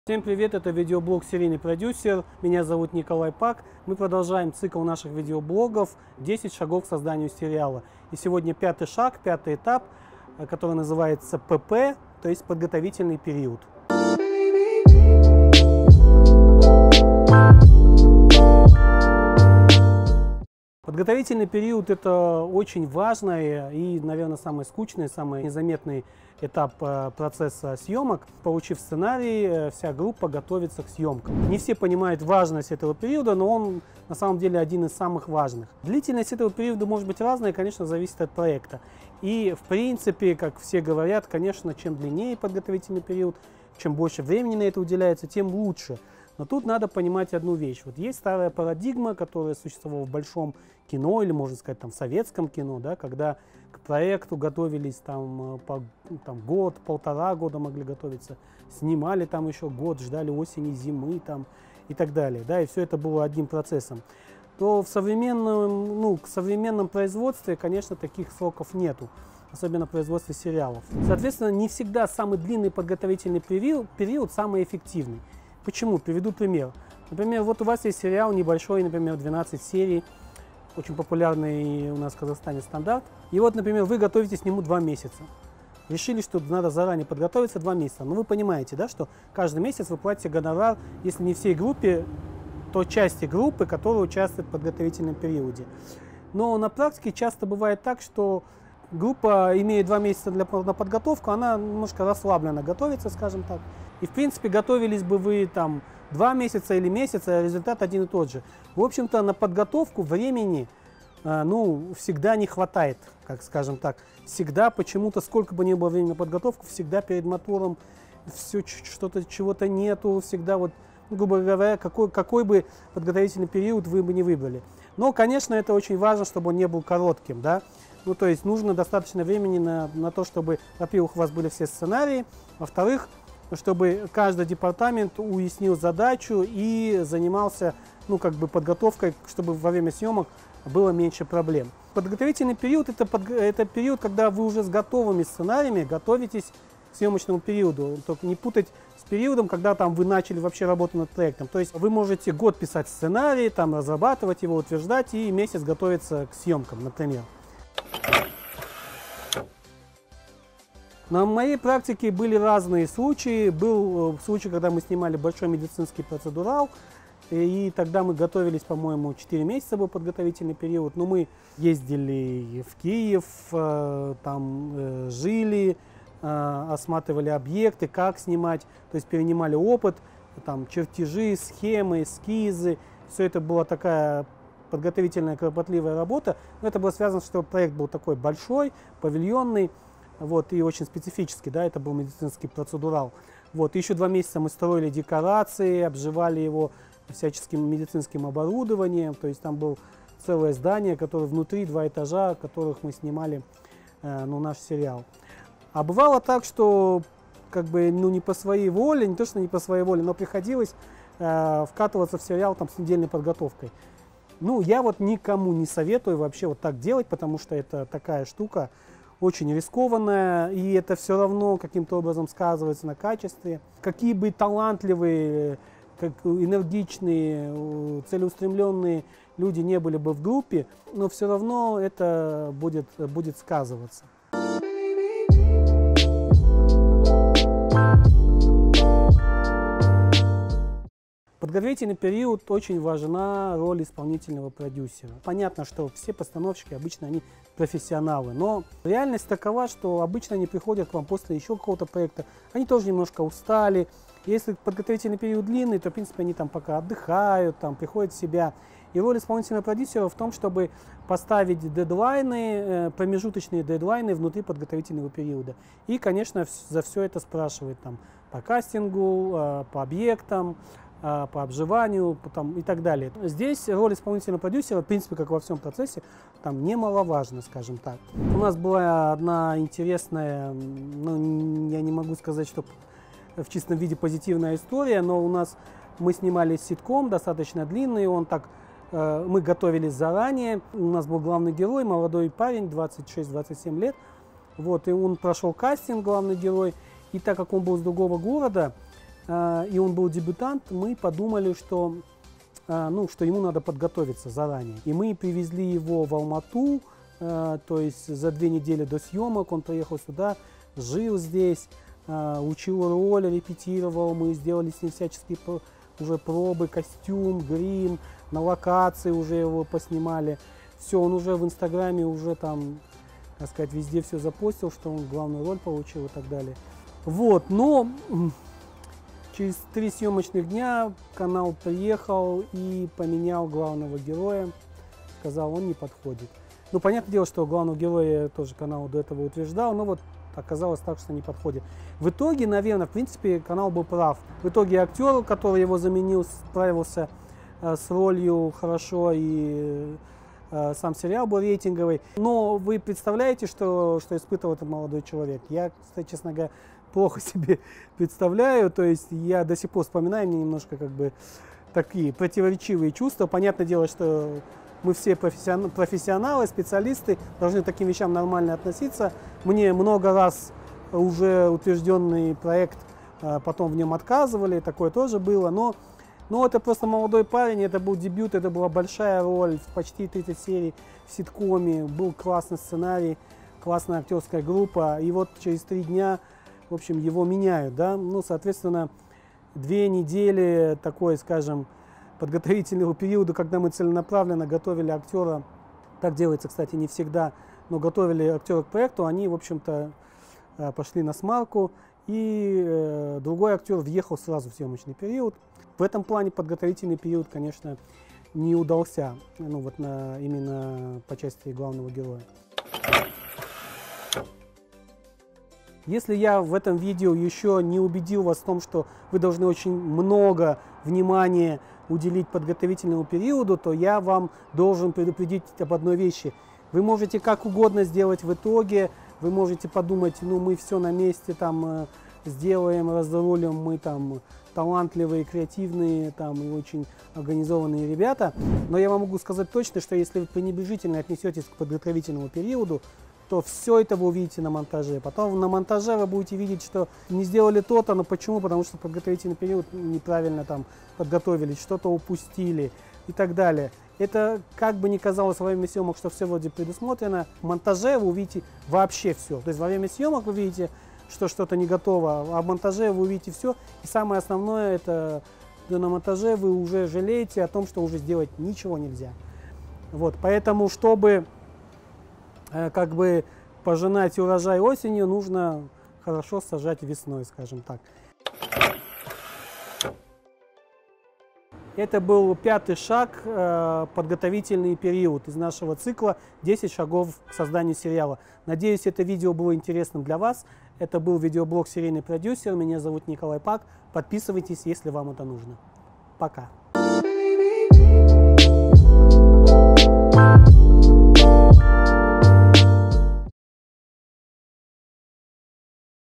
10 шагов к созданию сериала. 5 шаг. ПП/Замена актера/Горький опыт
В этом выпуске разбираем подготовительный период к съемкам сериала.